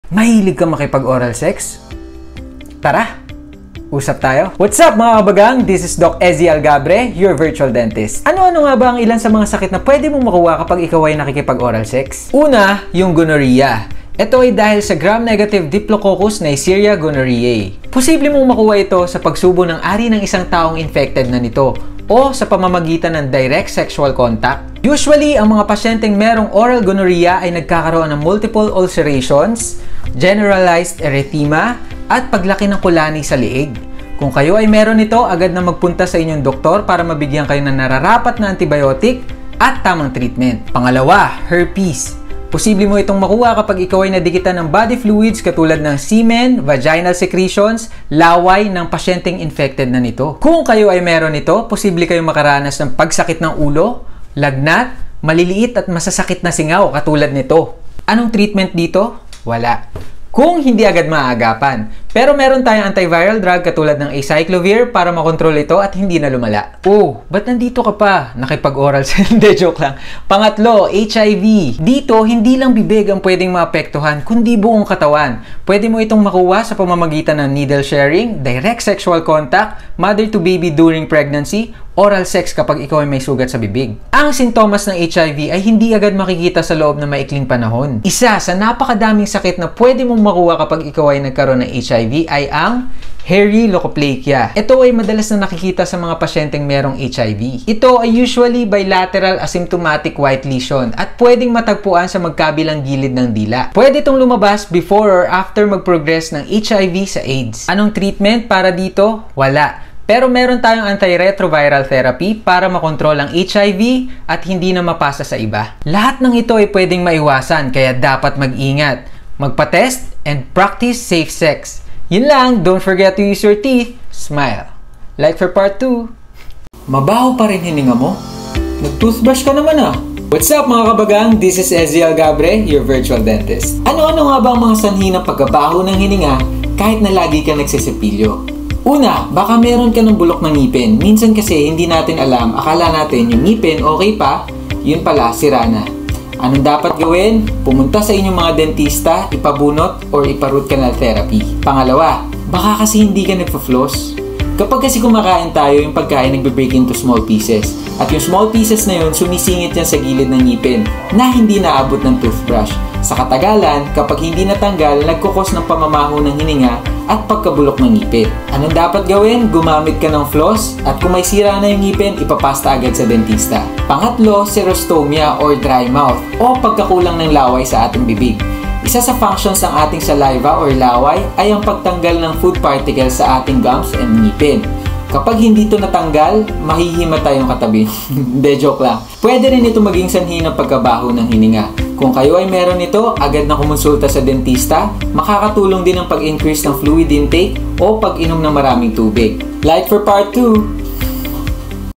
ORAL STD | BAD BREATH
Mahilig ka magoral? Mabaho pa din hininga mo kahit nagsisipilyo?